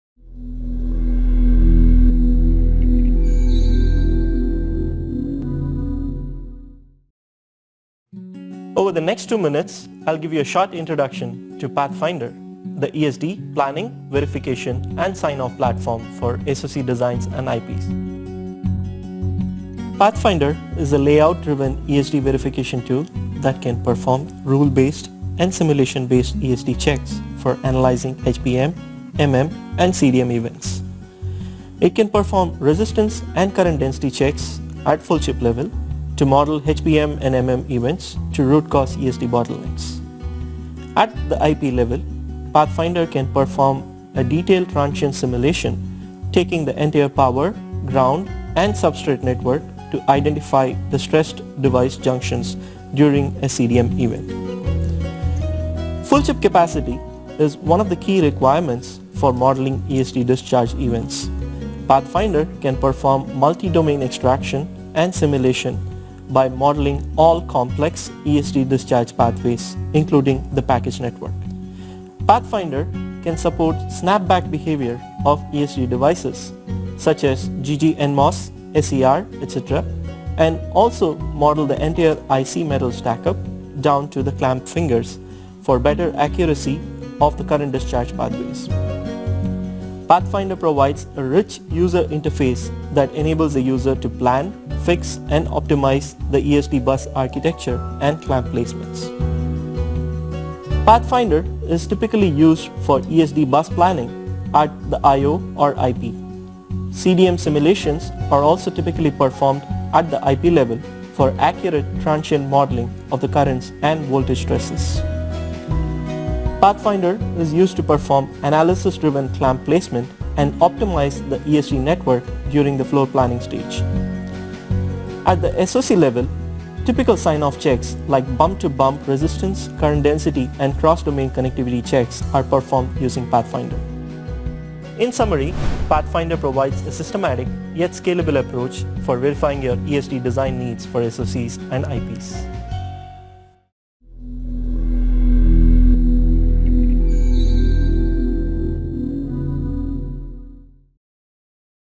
ANSYS PathFinder [Overview]
ANSYS PathFinder is an electrostatic discharge (ESD) planning, verification and sign-off solution for full-chip SoC and IP designs. It is applied in layout and circuit-level analysis to identify and isolate design issues that can cause chip or IP failure from charged-device model (CDM), human body model (HBM) or other ESD events. A number of foundries have certified the technology for its ability to perform accurate interconnect parasitic extraction, ESD simulation and electromigration (EM) or current-density rule handling.